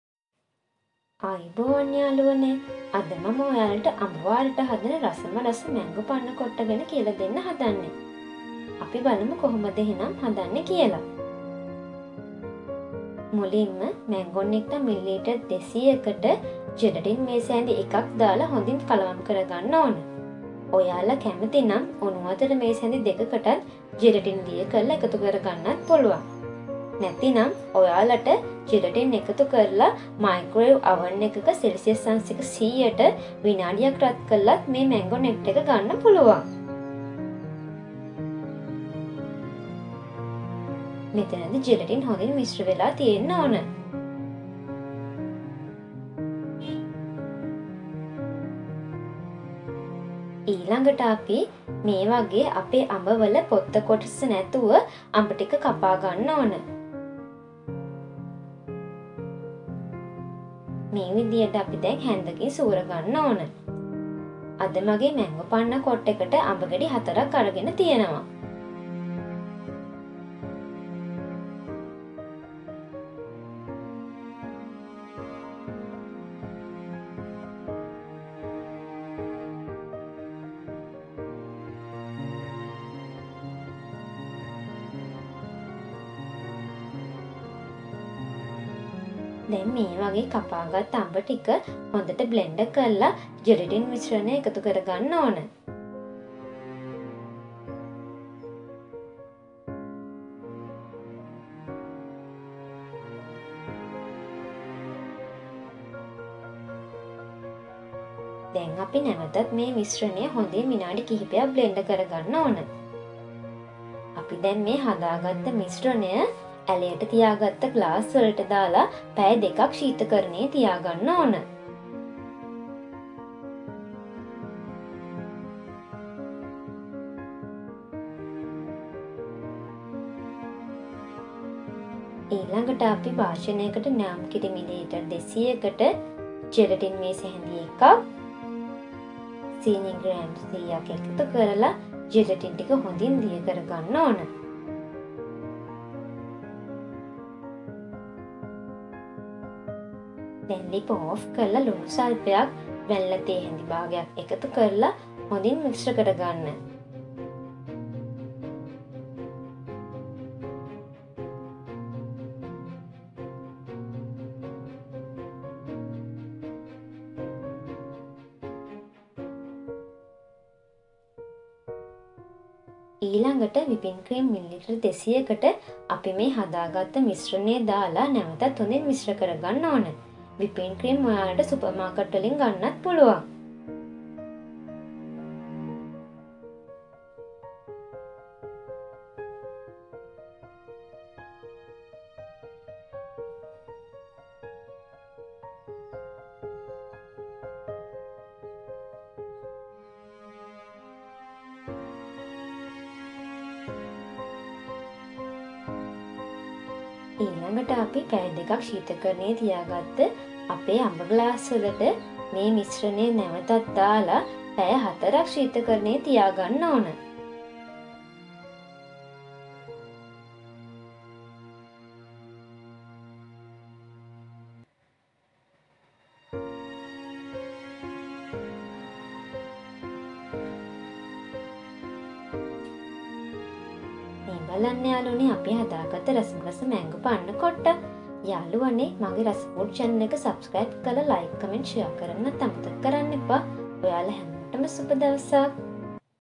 Mango Panna Cotta Mango Panna Cotta Recipe How To Make Panna Cotta@tharudiary8360
This video contain how we make Mango panna kotta easier as sri lankan style.This may teach you a taste mango Panna kotta recipe quickly can make for your dessert